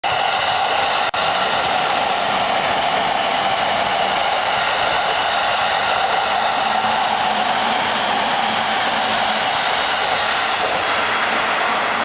【JR東日本】京浜東北線209系0番台 鶯谷 通過
209系電車は、JR東日本の直流通勤形電車。
国鉄から大量に引き継ぎ、老朽化が進んだ103系の置き換え、および、一部は輸送力増強用などとして、1993年（平成5年）4月より京浜東北線・根岸線、南武線に本格投­入された。これまでの鉄道車両の製造・整備の方法を全面的に改めた新しい設計思想（バリューエンジニアリングの手法）が採用され、JR東日本では本系列以降の車両を「新系­列車両」として区分している。